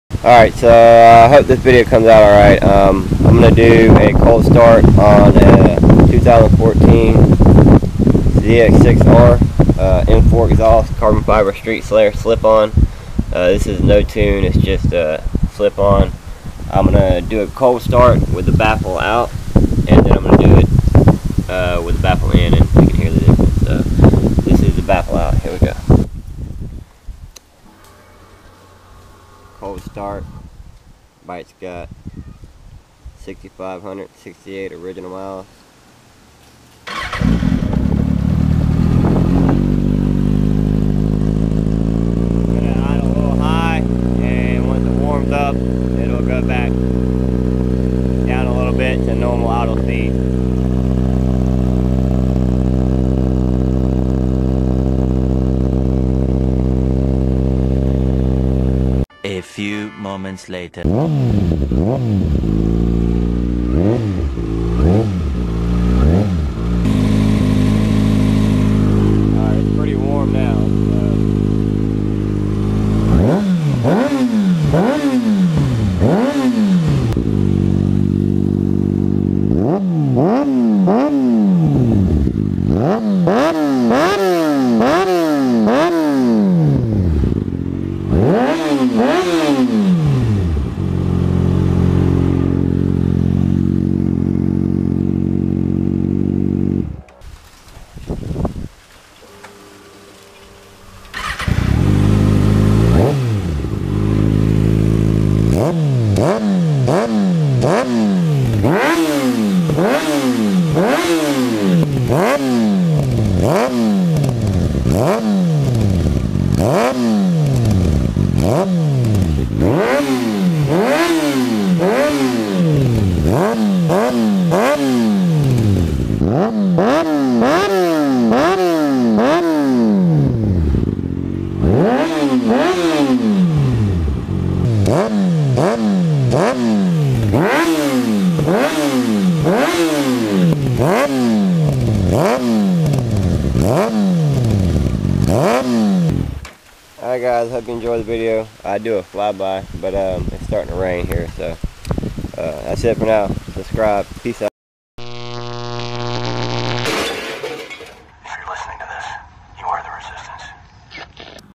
2014 ninja zx6r M4 carbon fiber slip on
With and without the silencer cold star m4 carbon fiber exhaust